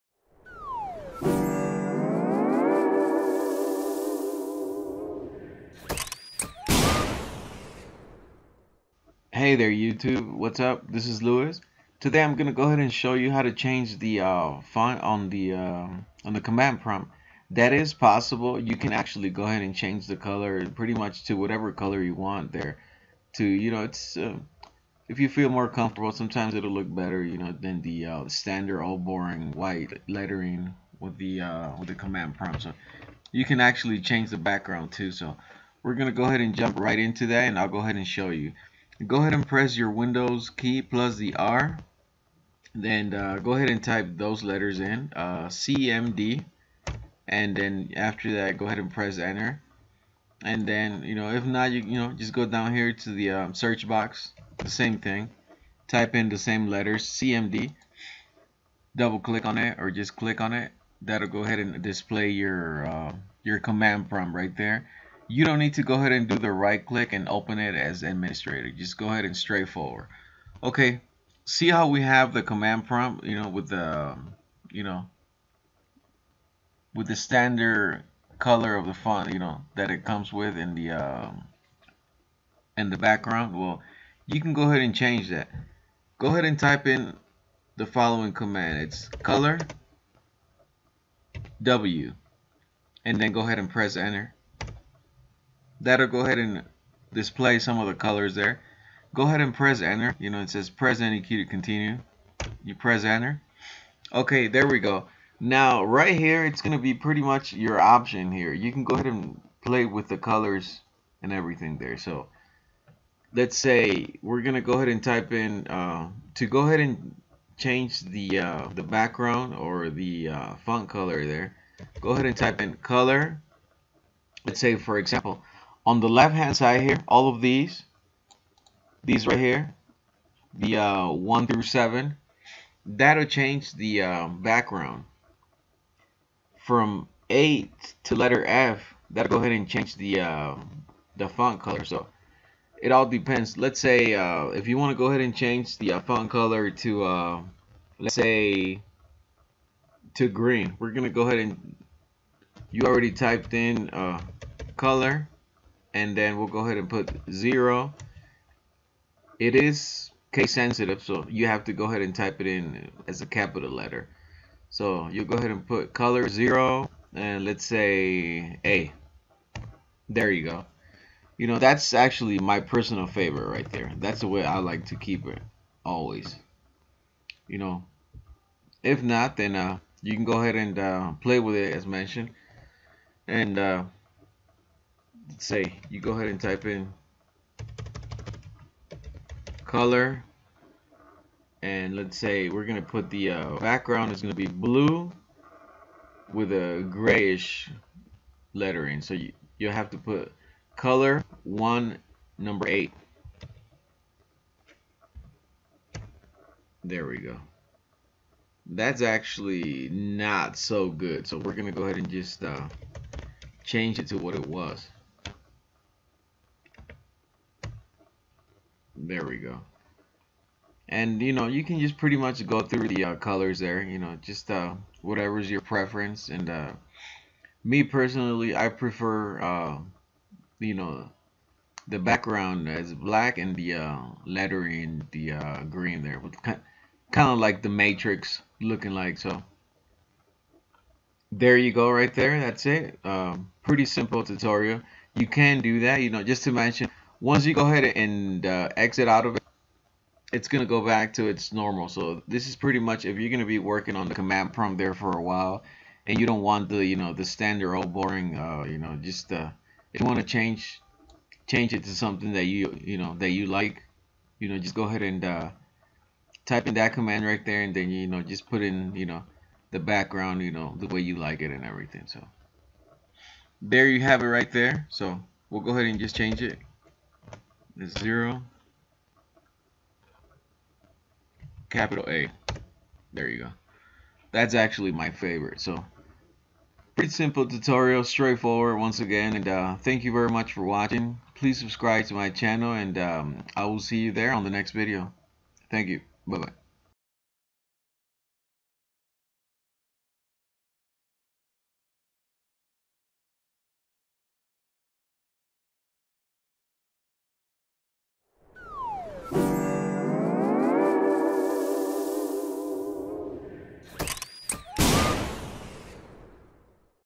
How to change your Font color on CMD as well as the background to the color of your choice!!!
This is a quick tutorial on how t change the Font and the background on the CMD, You can personalize the color without having your eyes hurting due to the old boring original default color.